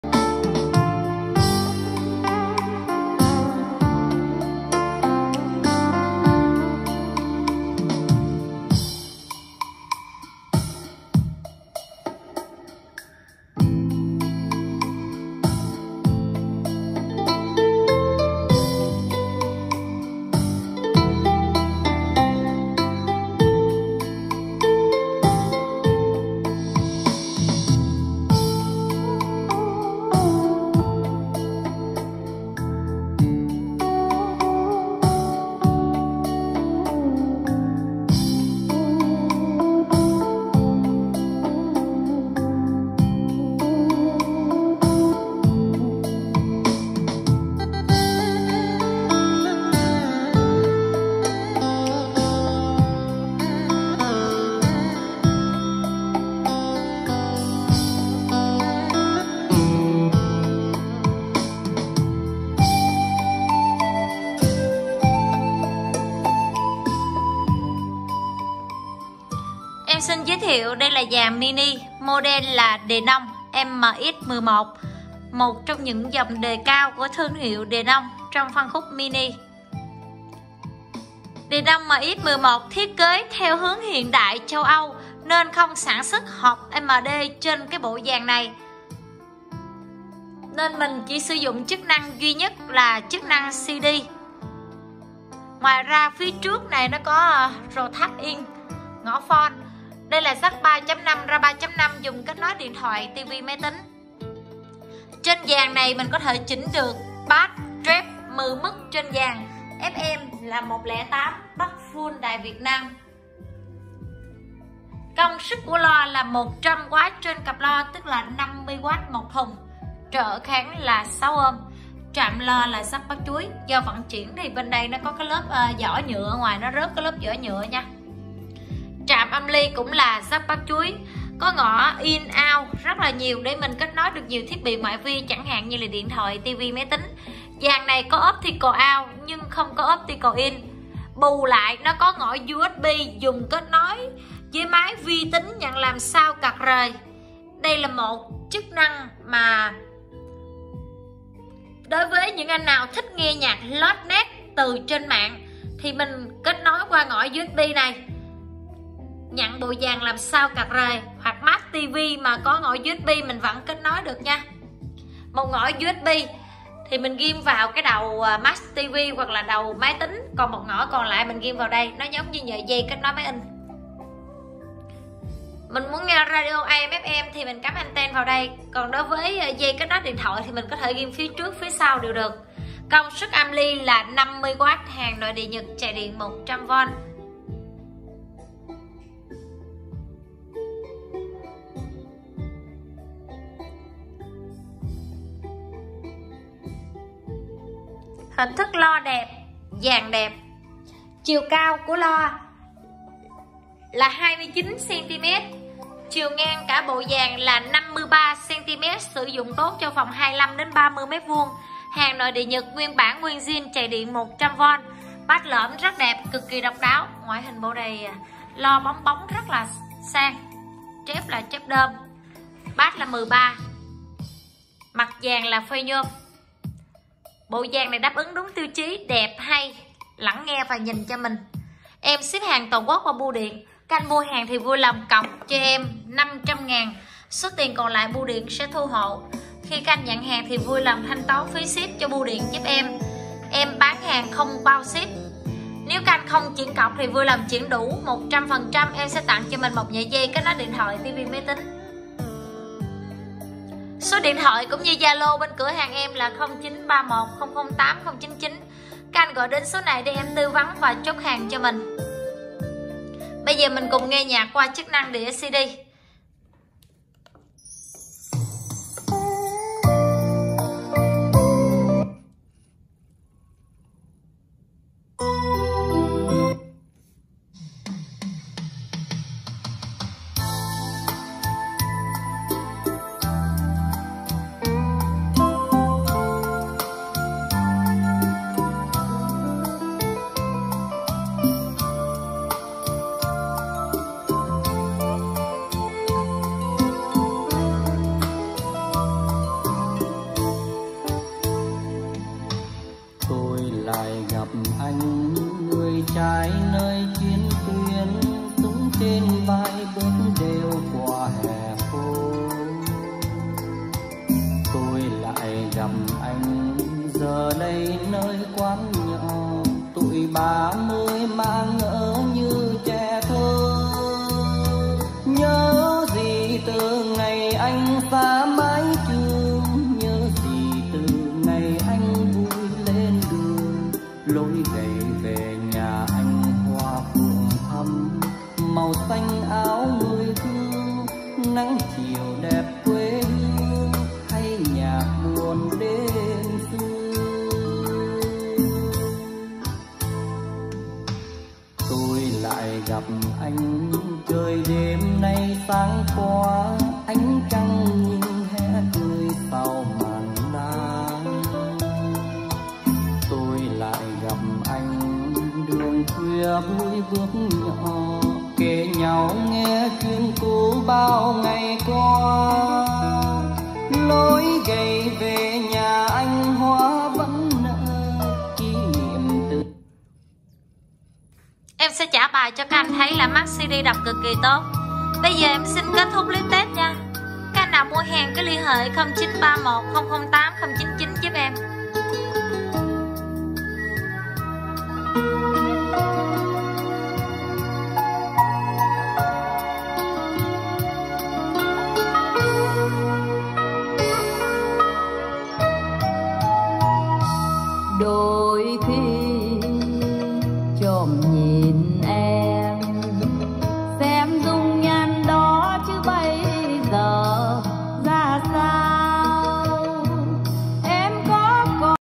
Đã bán--DENON MX11📌full chức năng CD/AUX/FM108Mhz/USB sound card ☎️0931.008.099(ZALO)
❌ Gía bán :  ❌❌cod cọc 500k thu hộ phần còn lại.

📞☎️KẾT BẠN ZALO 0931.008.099 ĐỂ CHỐT HÀNG
♻️♻️Denon MX11 - SIÊU mini. Hàng nội địa nhật, chạy diện 100v.

👏👏Chức năng: CD, USB SOUND CARD, FM108Mhz , AUX KẾT NỐI ĐT TIVI MÁY TÍNH.
📌DENON D-MX11 thiết kế theo kiểu hàng xuất Châu Âu nên không có MD .

👉Mặt phay nhôm , vuốt cạnh mềm mại .

👉Chỉnh Bass , Treb 10 mức ,chỉnh luôn S. Bass và direct trên dàn.

👉Bắt tốt FM việt nam, full tần 108Mhz.

👉Có chức năng PC-USB ( làm sound card cho PC ) , ai nghe nhạc từ Latop và PC thì chức năng này rất ý nghĩa và đáng quan tâm .

📌Loa zin DENON MX33, dòng loa bass lõm , bass 13cm, khỏi chê vế chất lượng âm than em này.

                         LOA:100W/1 CẶP, BASS 13cm

🔺Chiều cao 28cm, dài 55cm
📞☎️ SĐT NV BÁN HÀNG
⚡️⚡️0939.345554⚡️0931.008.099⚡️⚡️